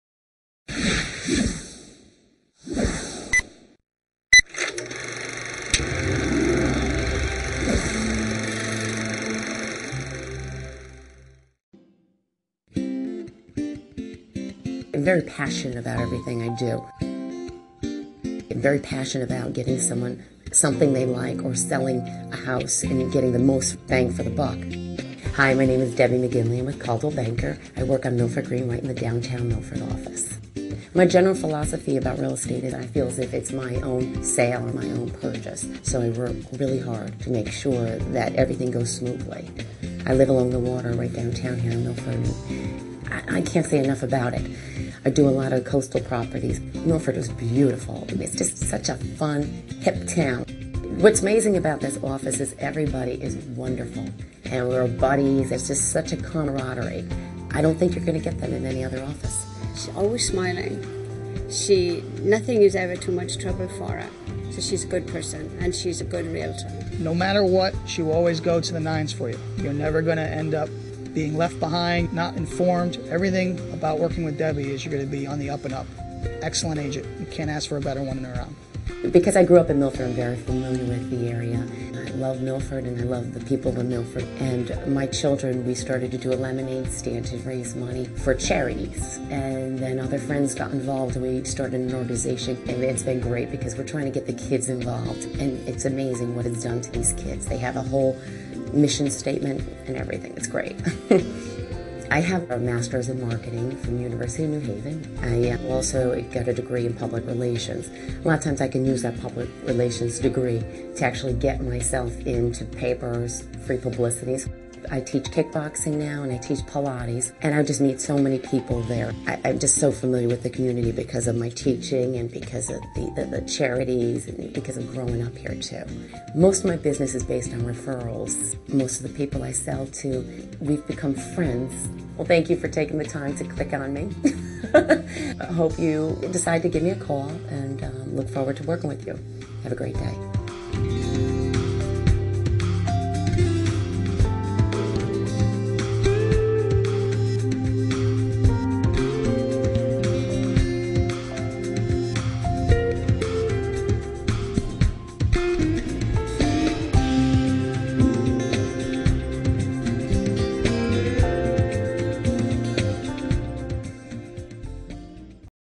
Debi McGinley Video Resume
We would like to introduce Debi McGinley of the Coldwell Banker Residential Brokerage Sales Office in Milford, Connecticut. Please enjoy the video.